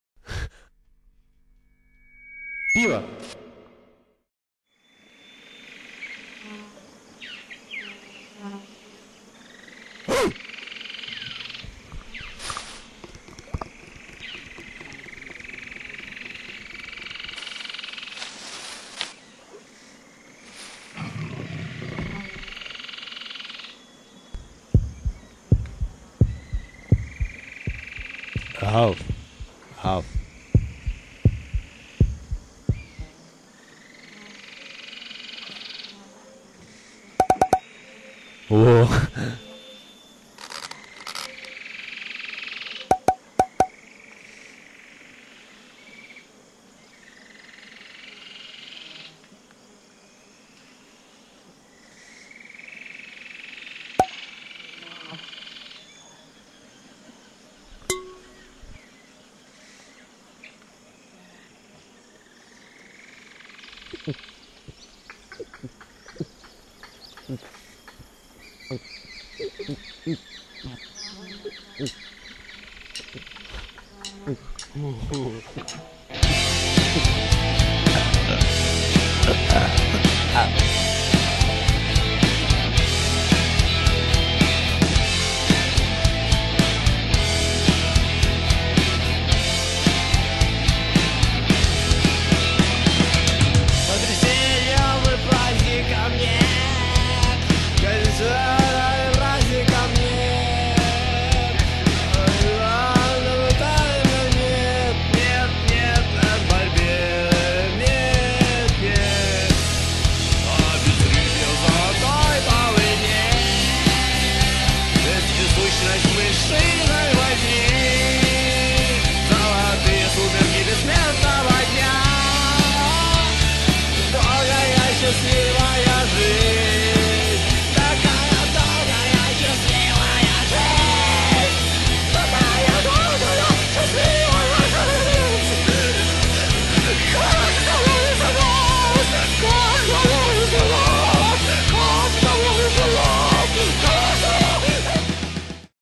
cartoon about beer in dugout
cartoon about beer in dugout and forest and dumb peoples with boromotuha. He drunk wery much vodka and sleep in snow pit